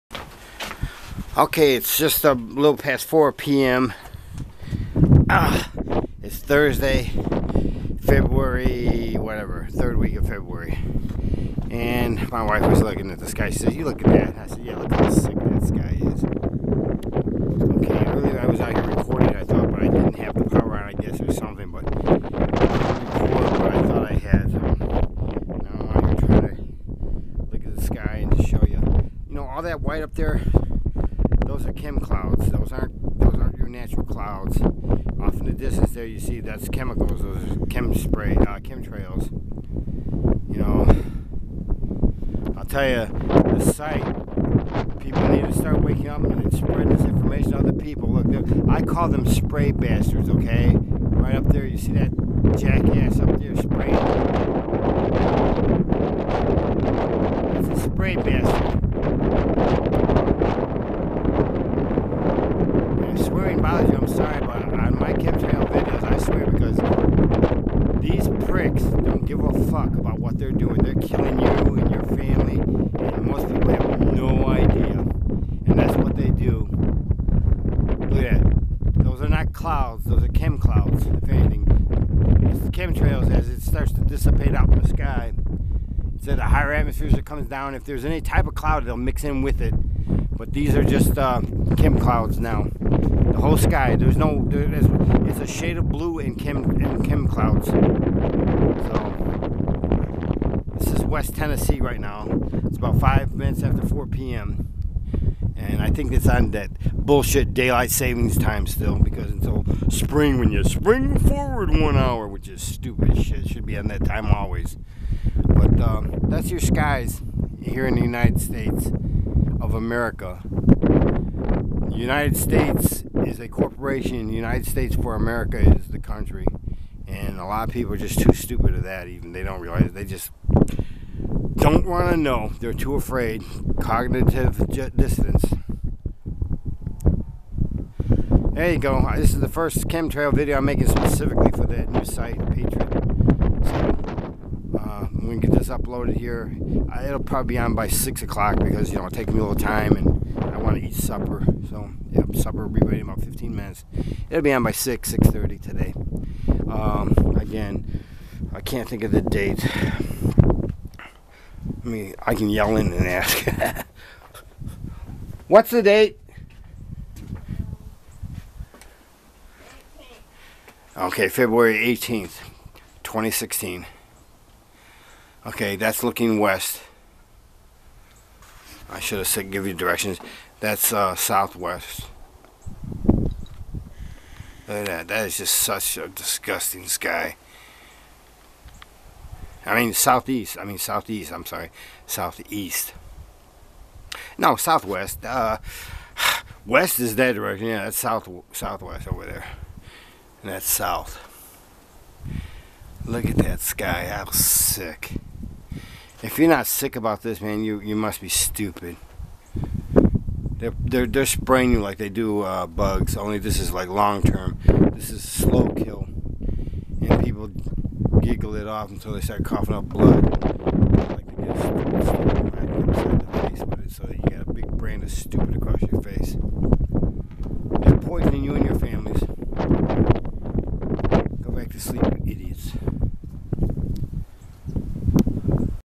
They just DON'T STOP 2016 02 18
As on 99% of my Chem-trail videos, * Swearing IS PRESENT. THESE...? "Globalists", they ARE quite MAD, Insane, OF COURSE "INBRED" over Centuries.They HAVE planned to "Take it", all of it. "THEY ARE SO INSANE... FOR POWER"- They want the planet, for "THEMSELVES"! There ARE records on ALL of this, generally on Alternative sites, people are ridiculed, call nuts, conspiracy kooks, as this is THEIR method to CONTROL the MASSES ie DIVIDE & CONQUER! It is what HAS WORKED throughout HISTORY, BUT NOW...the MASSES ARE AWAKE, UNITING & "They see that" THEY ARE AFRAID because - there "IS" STRENGTH IN NUMBERS!